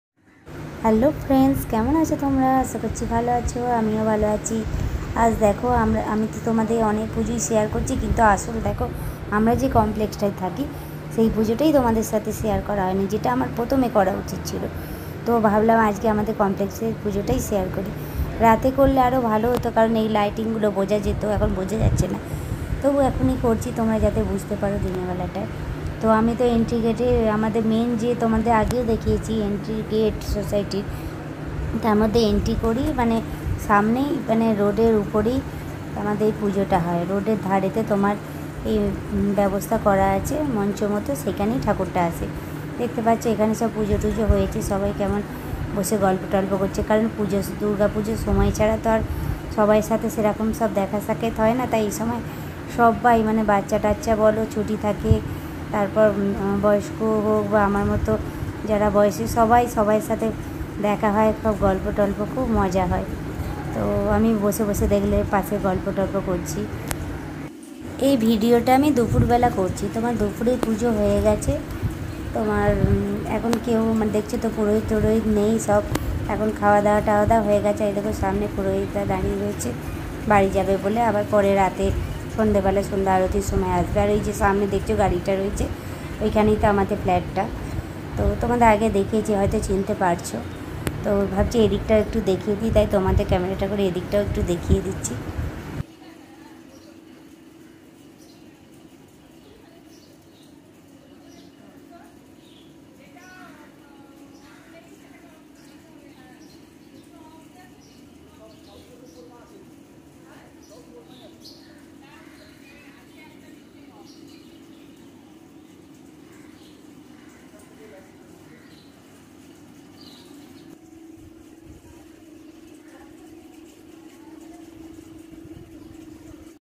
Amader Complex er Durga Pujo#আমাদের আবাসন এর পূজো /Durgapuja 2022#rainbowstyle
আজ আমি তোমাদের আমাদের আবাসনের দূর্গা পূজো শেয়ার করলাম।

my complex durgapuja
saltlake AN residency durga puja 2022